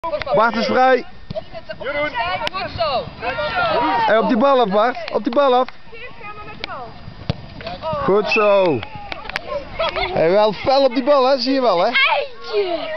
Bart Sabel 1-0 (1 sep 2007: Rhode F10-Irene F5)
Bart Sabel scoort de 1-0 tegen Irene F5. Betreft een fragment uit de wedstrijd pupillen najaar 2007, 2e klasse 218: RhodeF10-Irene F5 einduitslag 17-0, gespeeld op zaterdag 1 september 2007 te Sint Oedenrode.

rhode f10